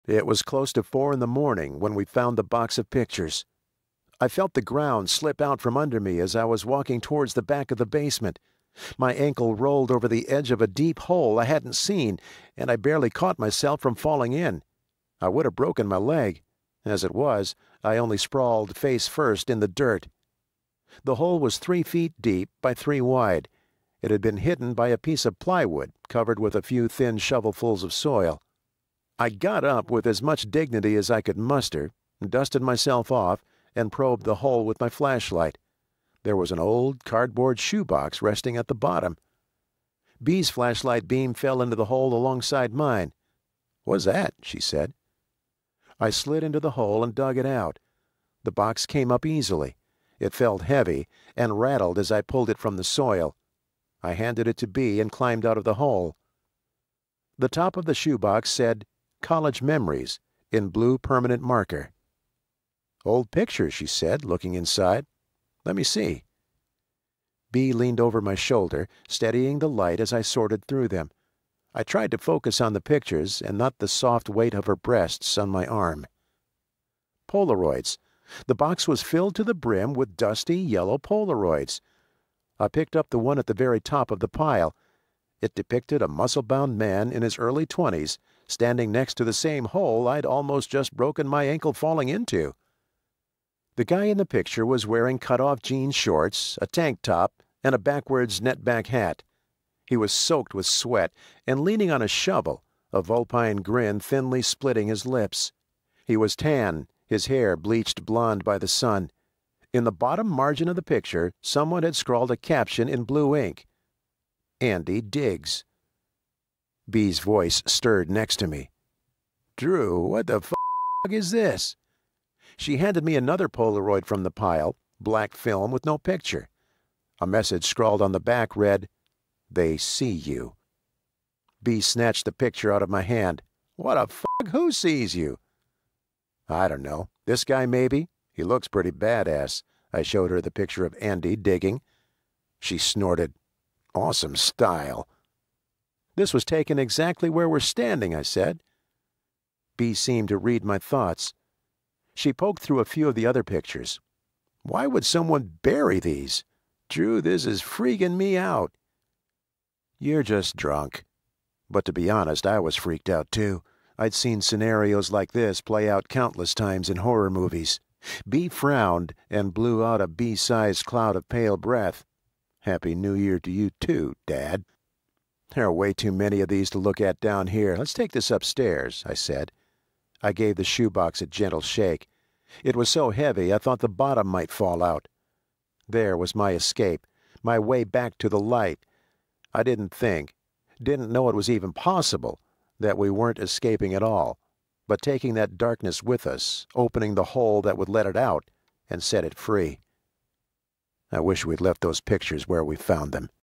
Audiobook Narrator Kevin Meyer LURK by Adam Vine
Audiobook narrator Kevin Meyer narrates LURK by Adam Vine.

The creepy story of college student Drew Brady who finds a box of old Polaroids buried under his house that can change to show him whatever he desires. Drew realizes there is something under the house that is manipulating him through the pictures, and it will drive him to do unspeakable evil if he doesn't look away.